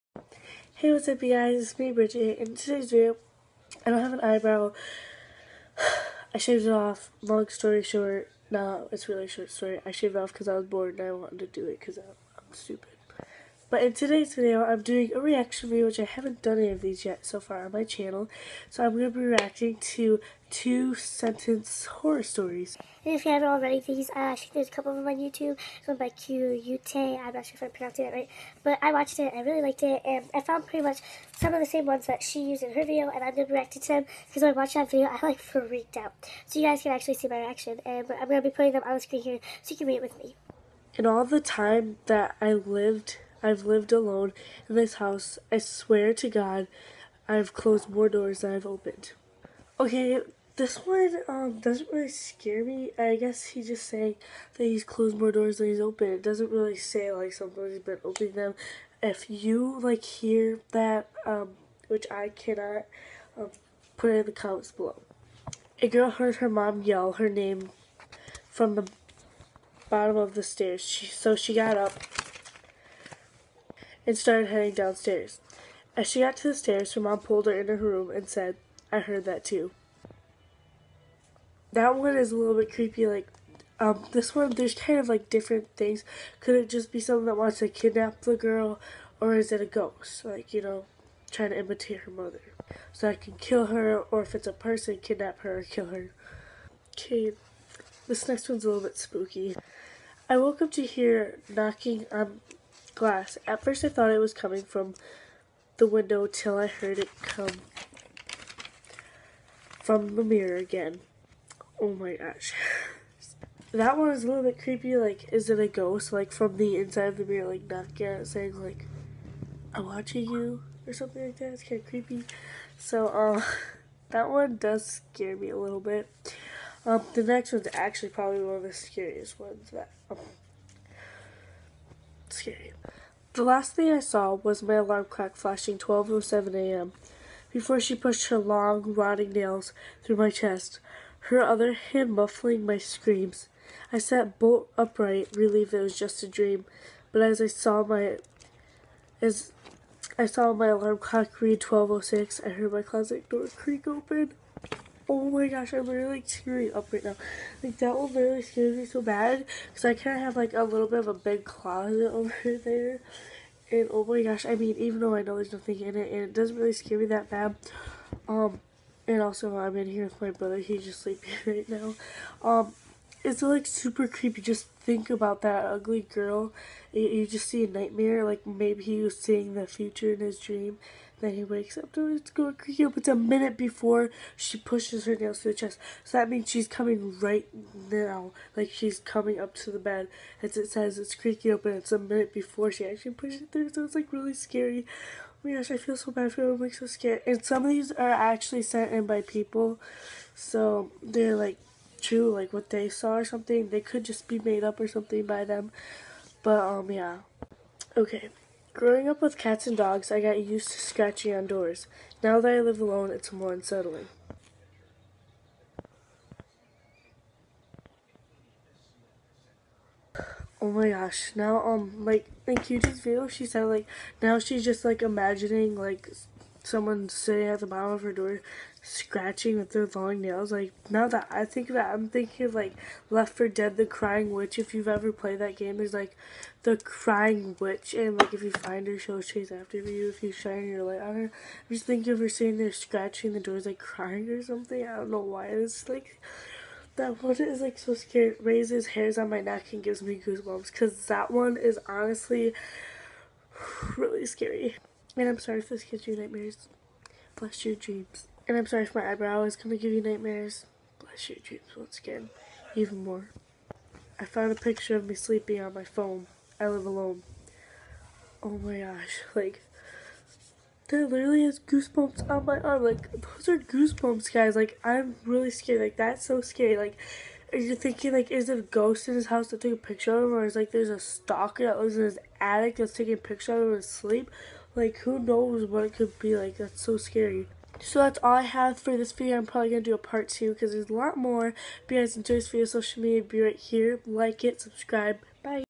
Reacting to 2 sentence horror story
Hey what's up you guys in today's video I am doing a reacting to scary stories into sentences These actually really scary!!!